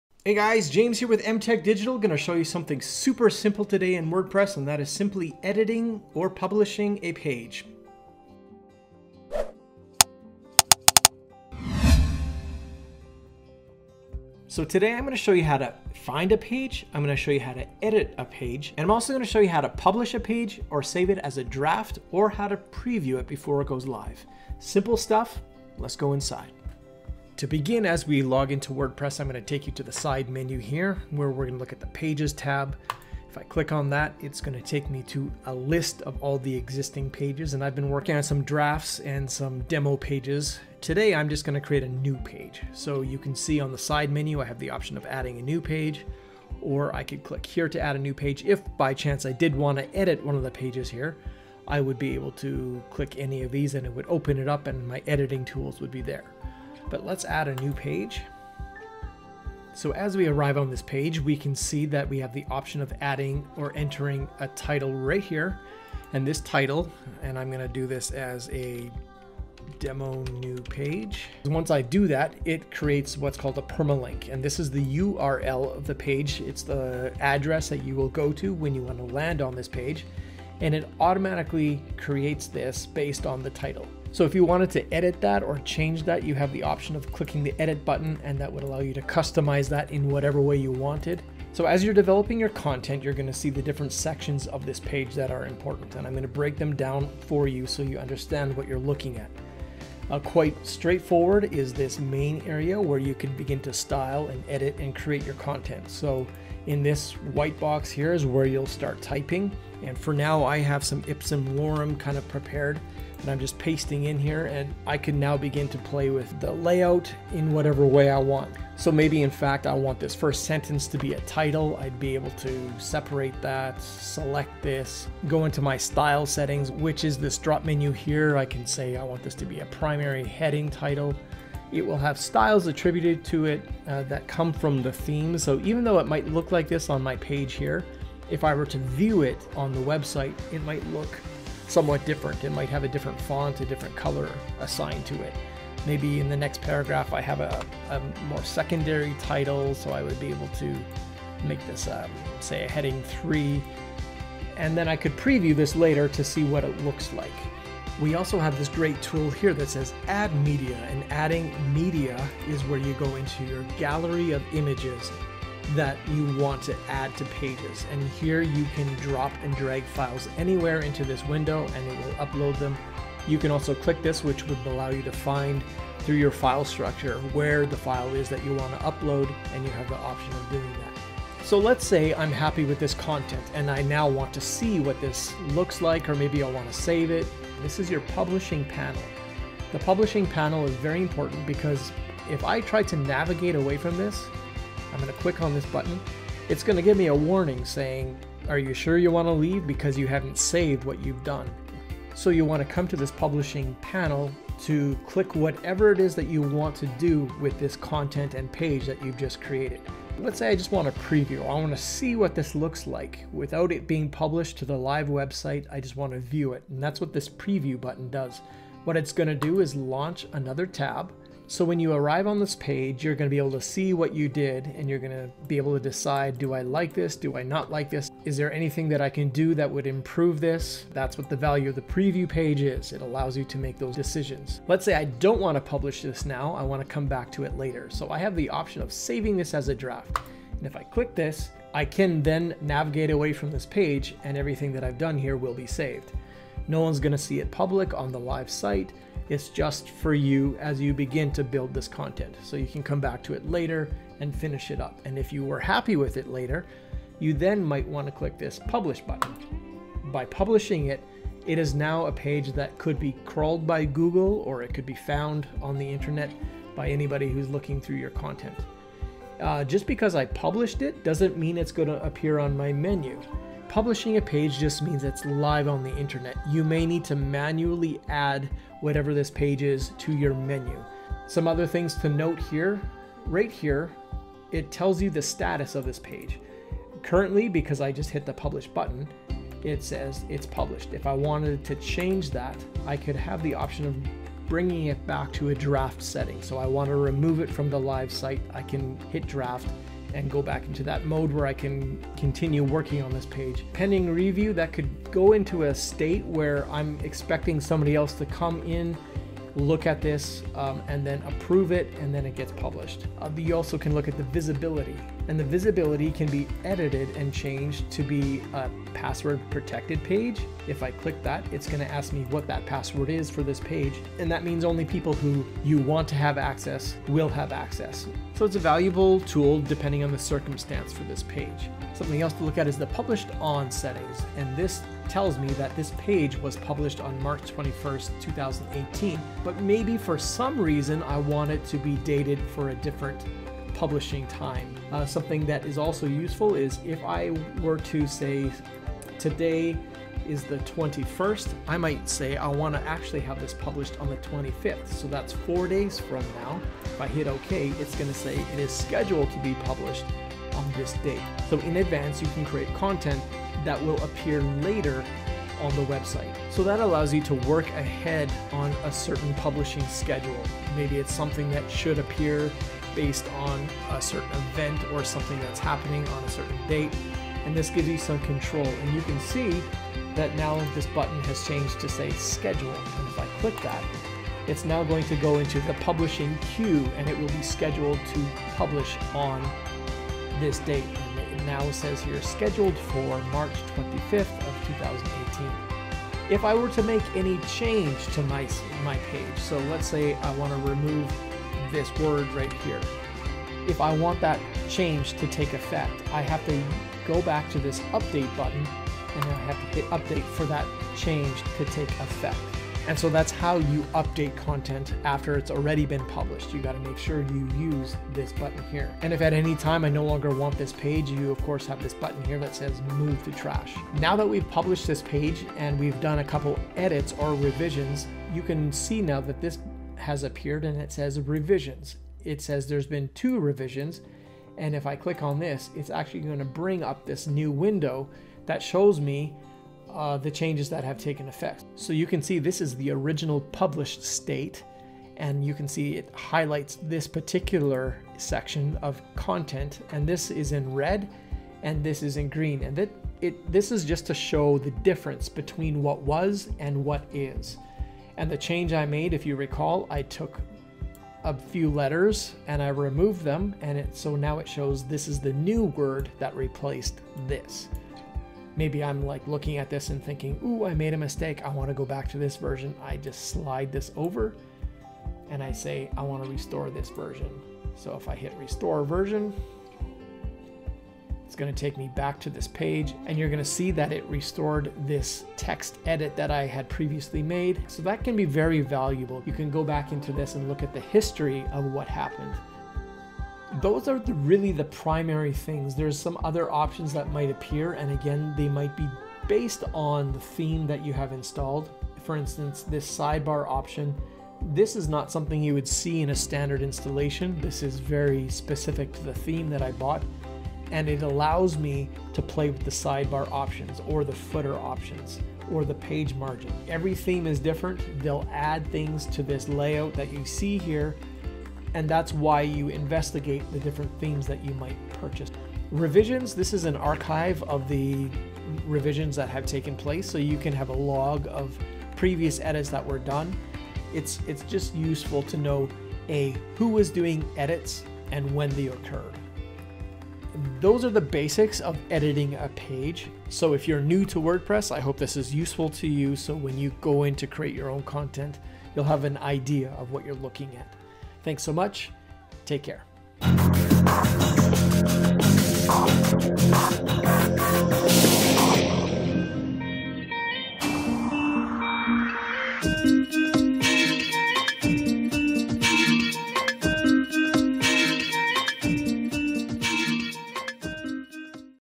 How to Create, Edit and Publish a Wordpress Page
Basic Wordpress help for the beginner showing how to create or edit pages and how to save as drafts or publish them on your website.